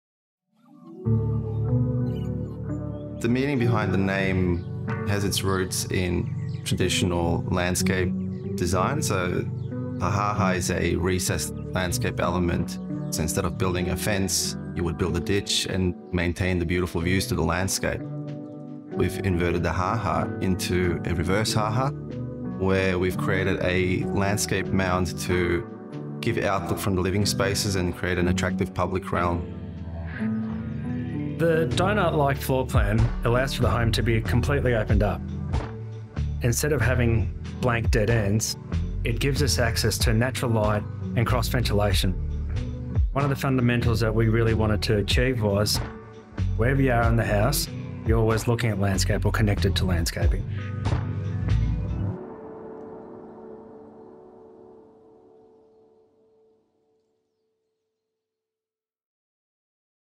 Ha Ha Haus by Figr | Brickworks X Est Living
Embedded within the verdant environs of Alphington, located on Wurundjeri Country, Ha Ha Haus is a notable architectural project that emphasizes the paramountcy of a close-knit familial abode that caters to frequent overseas visitors, intergenerational living, and transient occupancy.